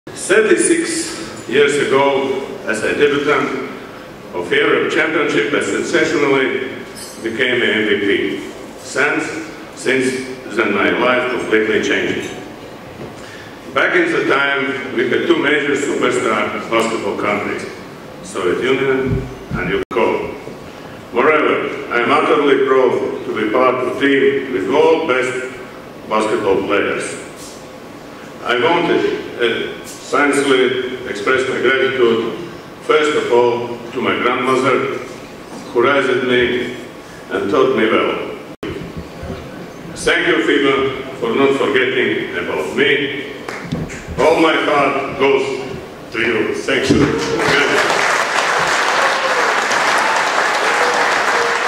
Valdis Valters' Induction Speech | FIBA Hall of Fame Class of 2017
Enjoy Valdis Valters' Induction Speech from the FIBA Hall of Fame Class of 2017.